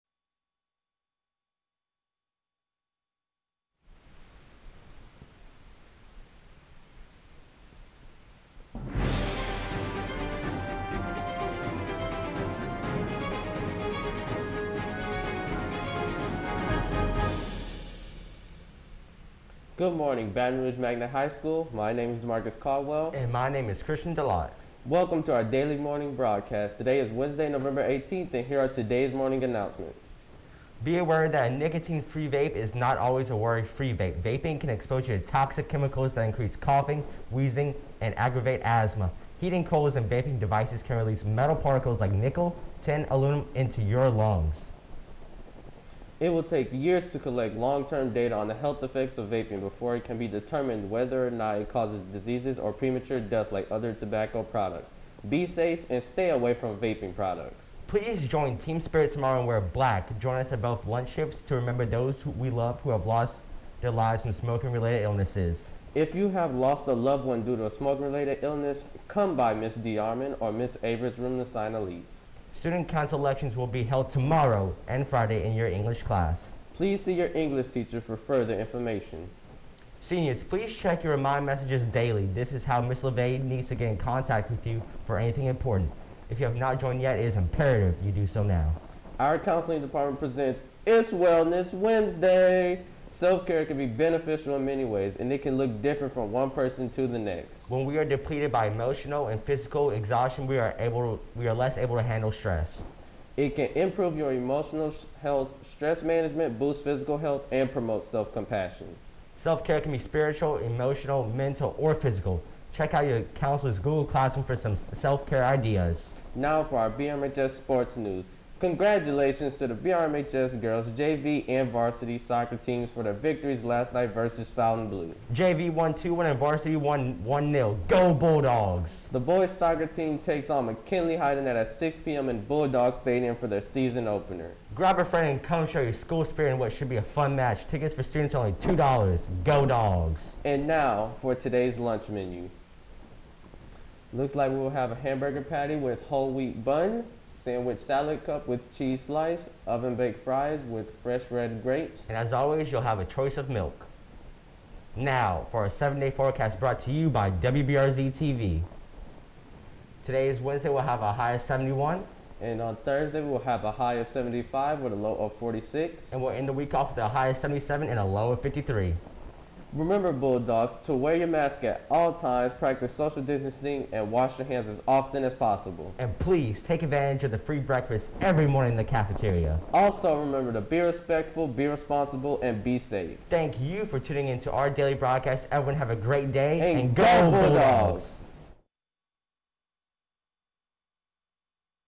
BRMHS Morning Announcements 11-18-20
Daily morning announcements presented by the TV & Film Production crew at BRMHS.